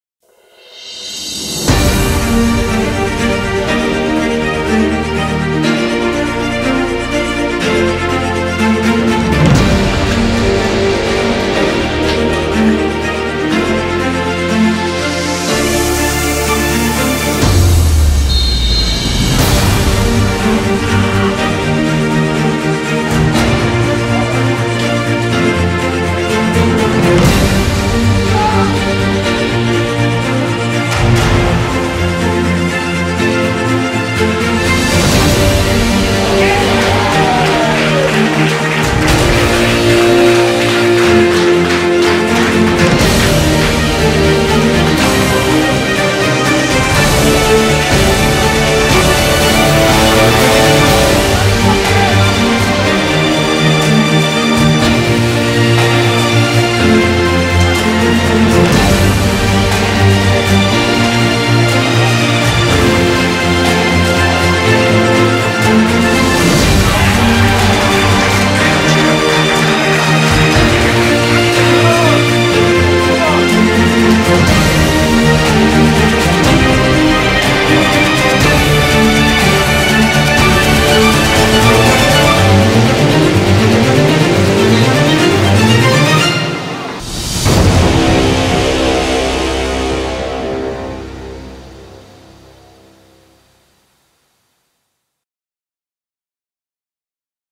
2024.05.01 A1: Polonia Bytom - TS Podbeskidzie Bielsko-Biała (gole)
Bramki z meczu 23 kolejki I ligi wojewódzkiej (Junior A1), rozegranego pomiędzy Polonią Bytom a Podbeskidziem Bielsko-Biała, zakończonego zwycięstwem bytomian 2:0.